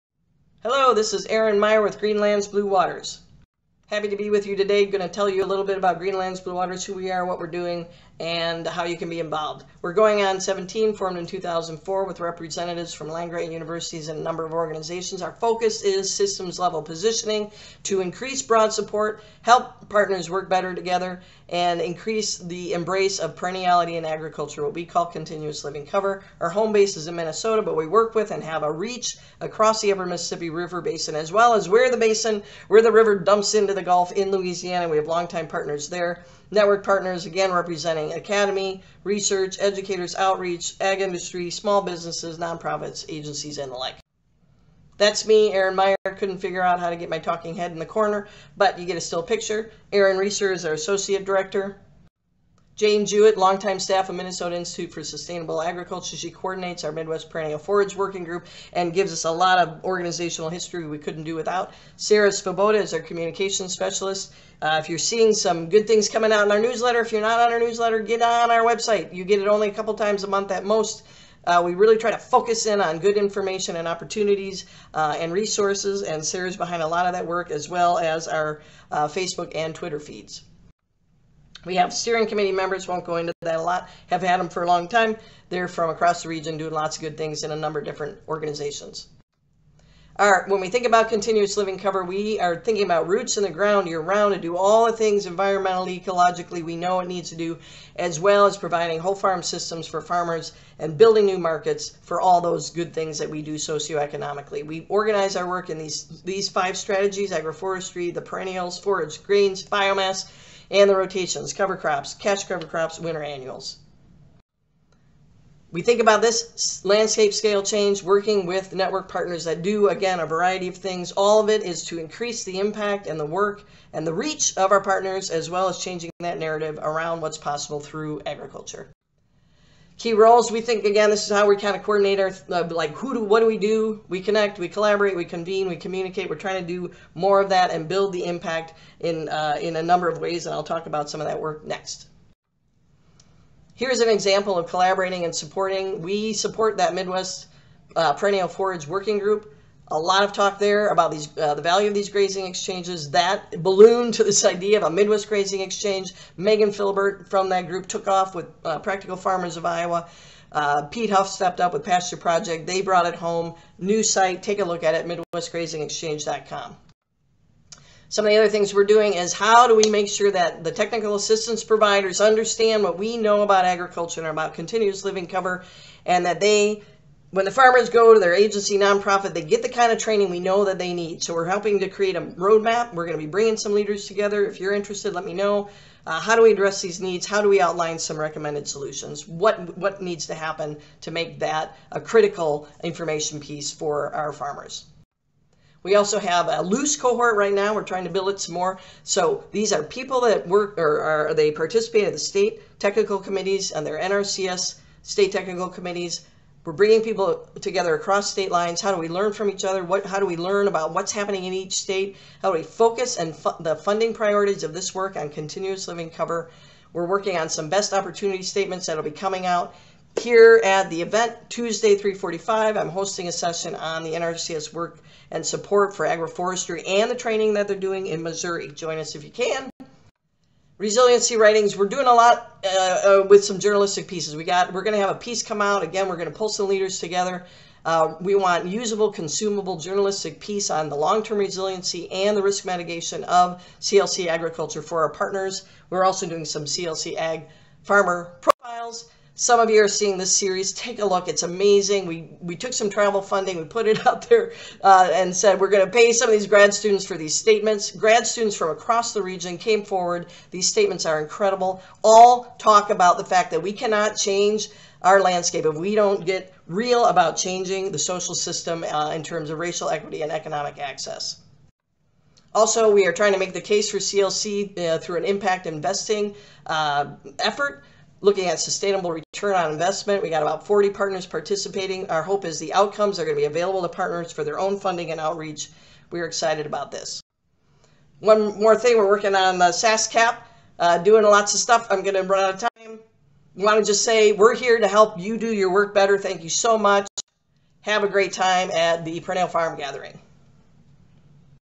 GLBW Intro Nutshell - Savanna Institute Perennial Farm Gathering 2020
Erin Meier provides a 5-minute overview of GLBW organizational purpose and current collaborative projects serving perennial partners